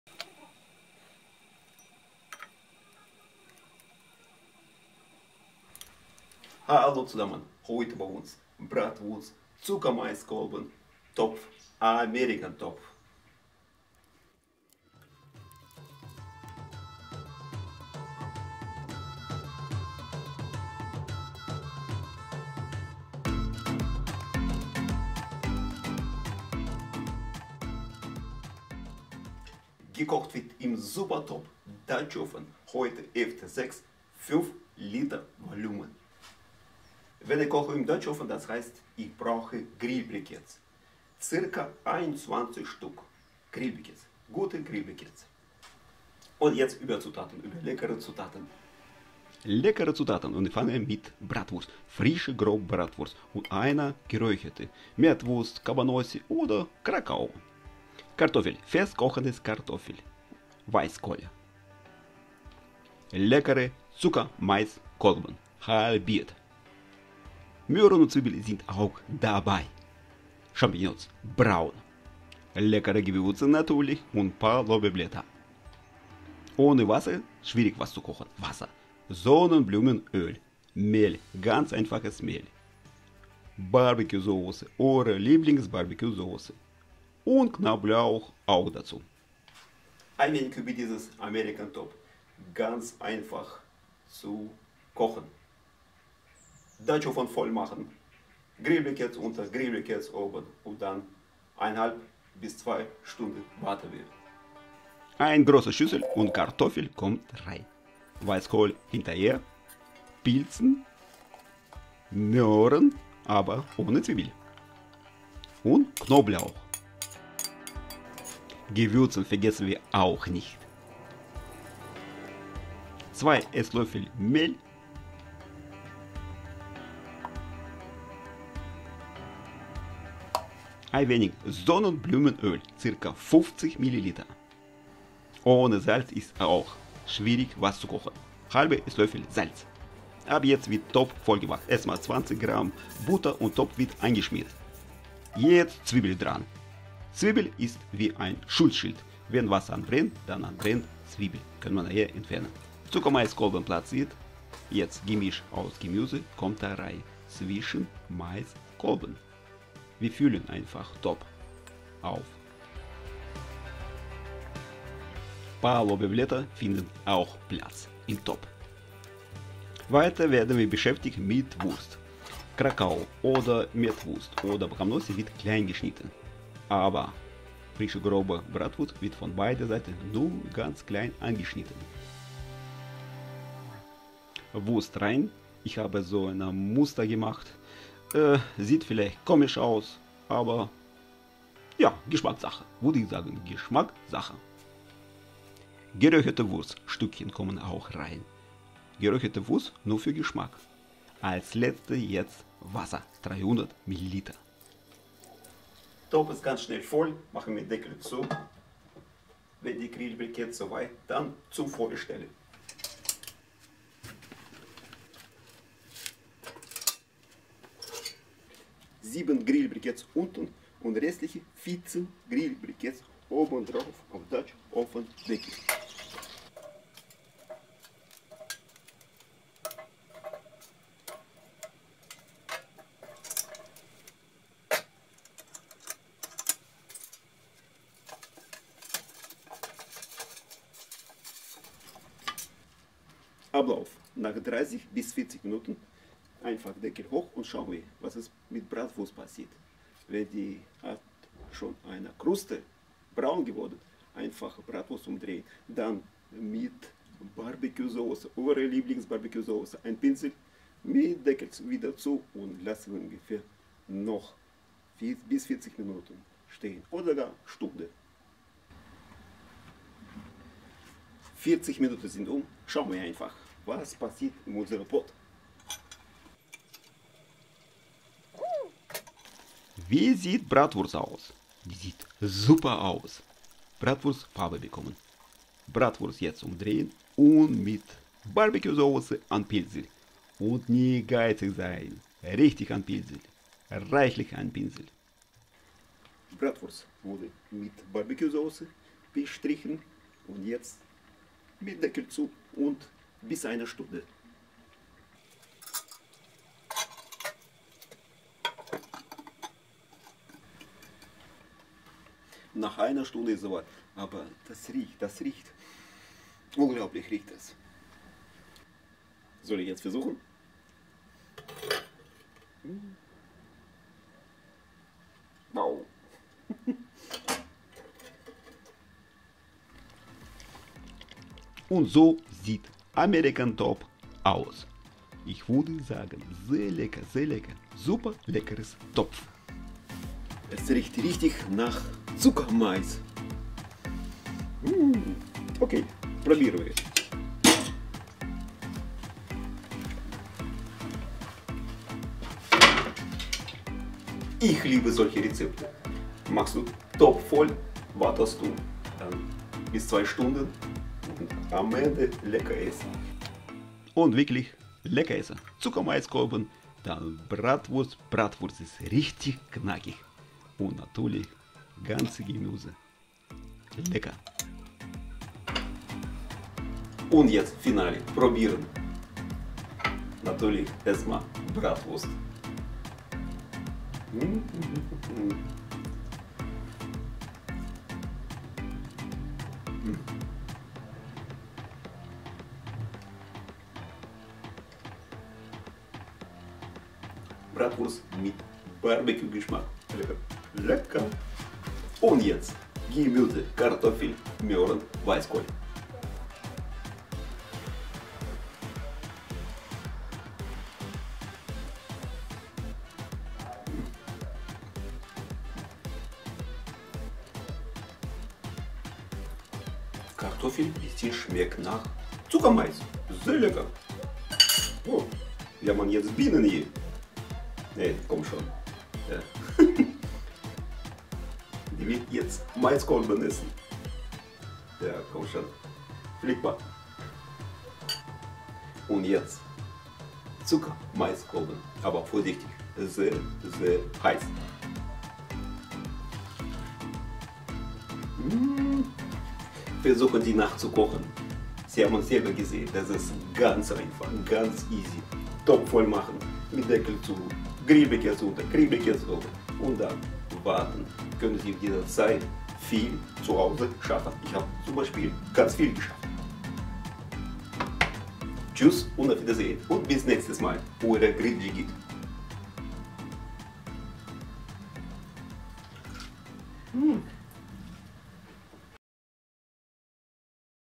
American Eintopf / Stew. Dutch Oven, Kazan Rezepte. One Pot
Dieser Dutch Oven Eintopf nach Amerikanischer Art ist ganz einfach, aber Mega lecker! Wer es nicht kennt... unbedingt nachkochen!
Dutch Oven Ft6-5 Liter Volumen, ca.21 Grill Brikets
Zutaten für ca. 8-10 Portionen:
1kg Bratwurst ( frische grobe)
1-2 Mettwurst (oder Kabanossi, Krakau)
4 Maiskolben ( halbiert)
1,5 kg Kartoffeln (festkochend)
ca. 0,3kg Weißkohl
2 Möhre
150g Champion
5 Knoblauchzehen
2EL Mehl
1/2 EL Salz
50ml Sonnenblumenöl
Gewürze:
1/2EL Paprika edelsüß
1/3EL Kreuzkümmel
1/2 Koriander
1TL Pfeffer schwarz
1/2 TL Chili
2 Lorbeerblatt
2  Zwiebeln
300ml Wasser
150g  BBQ Sauce